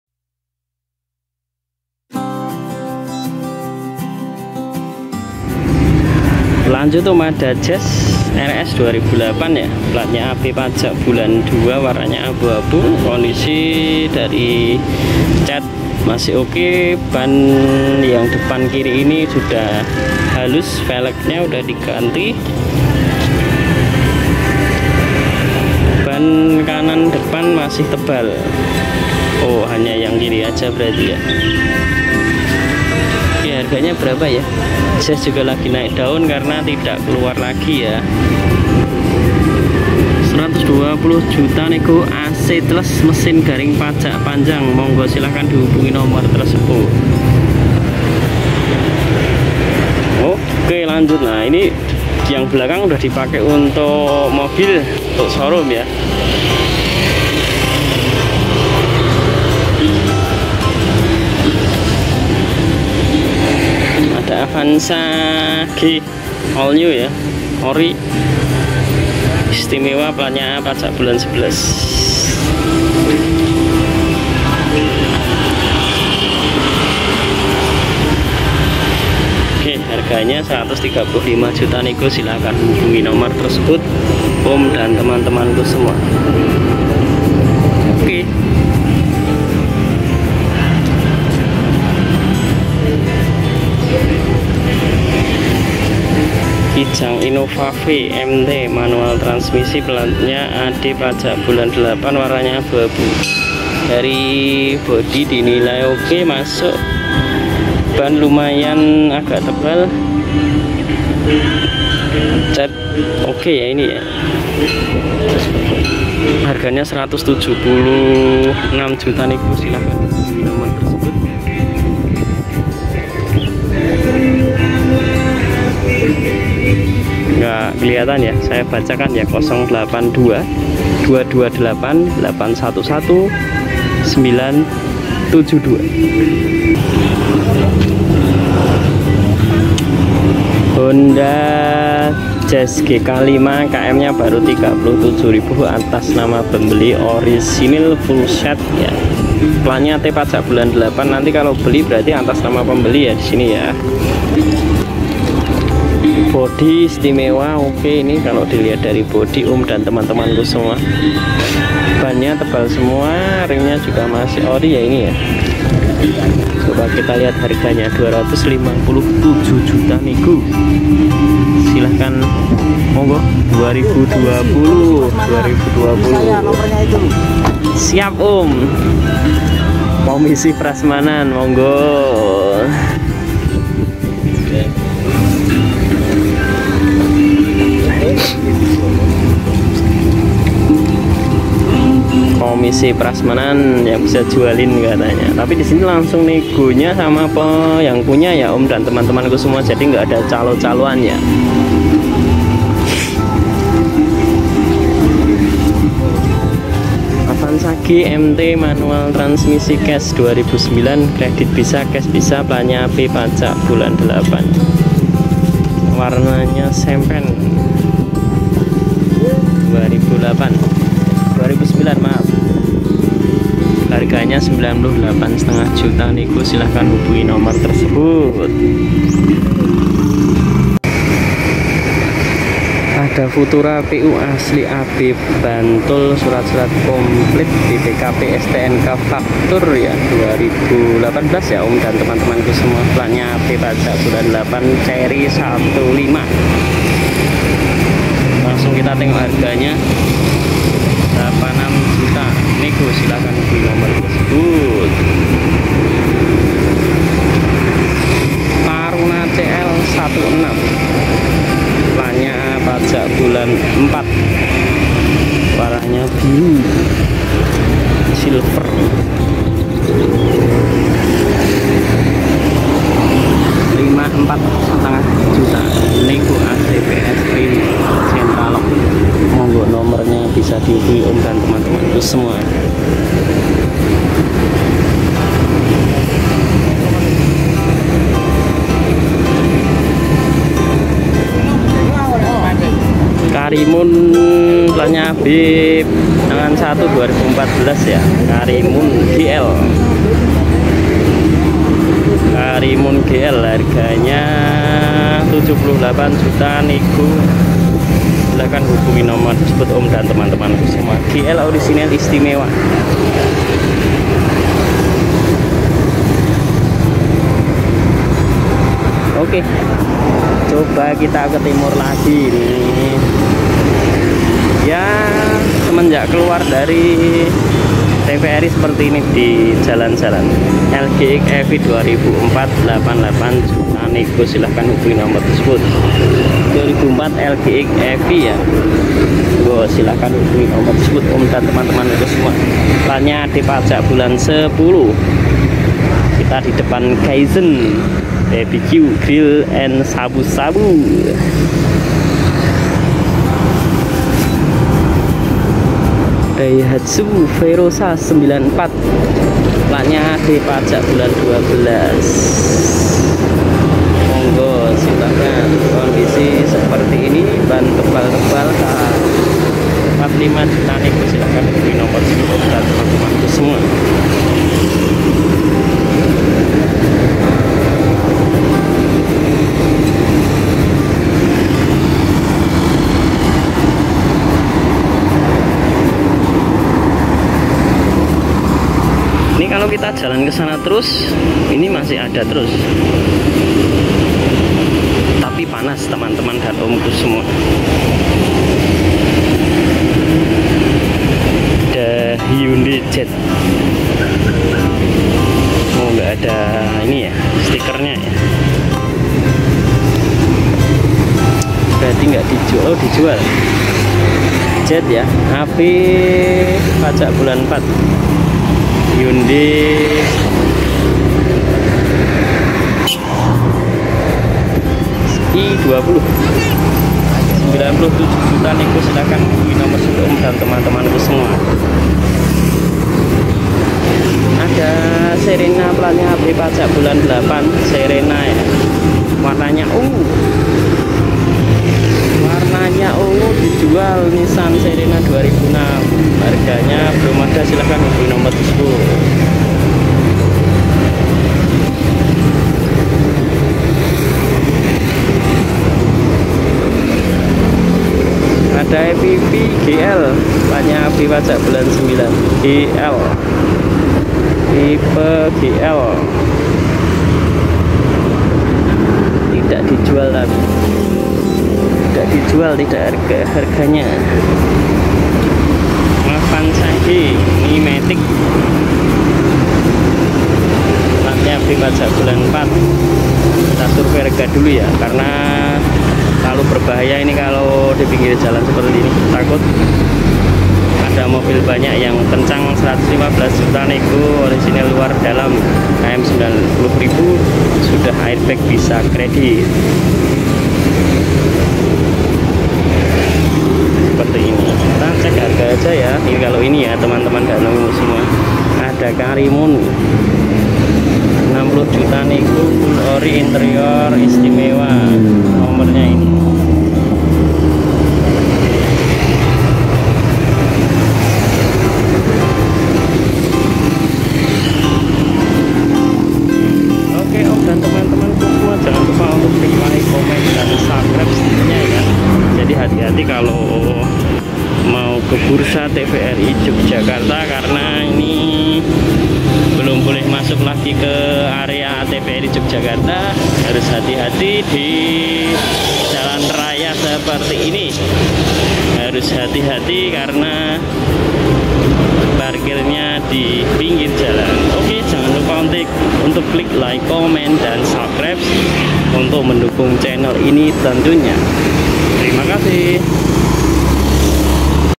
UPDATE - Bursa Mobil Murah di Jogja - Bursa TVRI Awal Tahun
Update Harga Bursa Mobil Bekas Depan TVRI Jogja terbaru

Terus dukung channel kami agar makin semangat lagi membuat konten konten terbaru.

k cunk motor terkini, zahra motor klaten, atmajaya motor
cahaya motor,